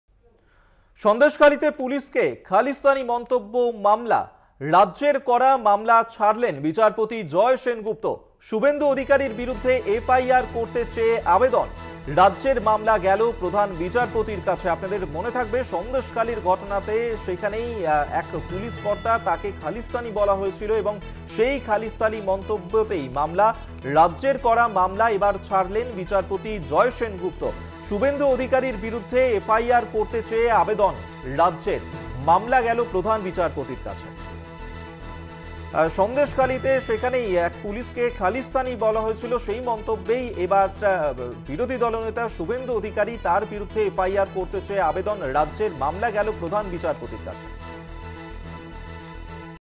Suvendu Adhikari | শুভেন্দুর 'খালিস্তানি' মন্তব্যে বিতর্ক, FIR সংক্রান্ত মামলা থেকে সরলেন বিচারপতি
website link for this copy :
@Kolkata Tv